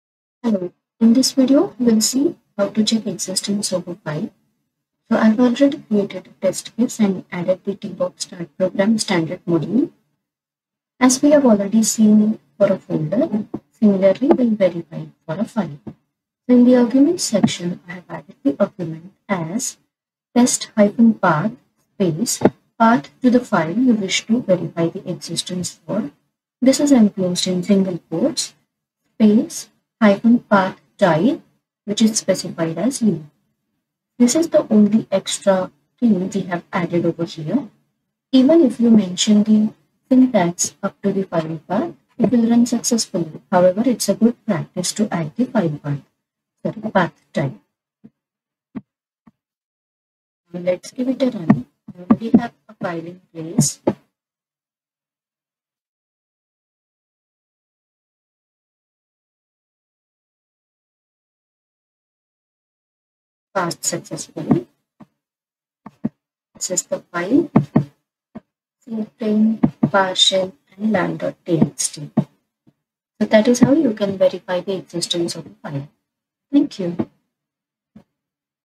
How to Check existence of a File using PowerShell with Tricentis Tosca?
In this video we'll see how to verify existence of a file in a folder using PowerShell with Tosca.

Syntax -
Test-Path 'Path' -PathType Leaf
It will work without adding the -PathType however, its a good practice to use it as we are verifying existence of file which resides in a folder.